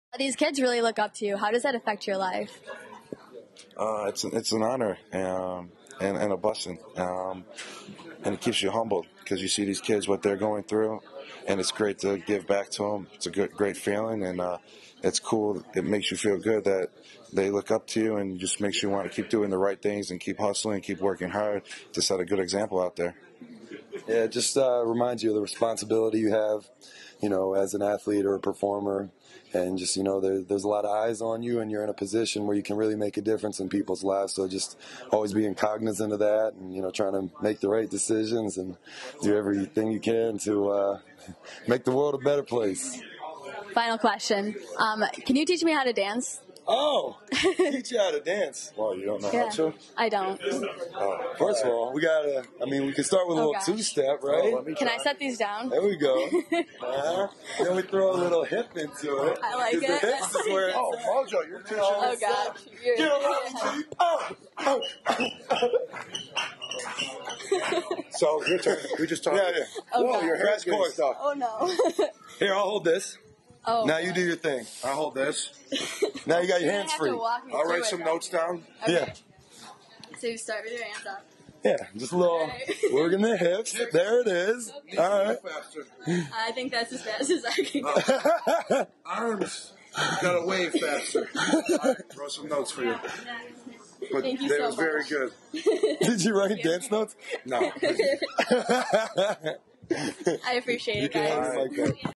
Dancing (and shopping) with Rob Gronkowski & Mojo Rawley
New England Patriots star Rob Gronkowski and rising WWE wrestler Mojo Rawley took kids battling cancer shopping for hats. Interview by Hailey Rose Gattuso at the New Era flagship store in Buffalo, New York.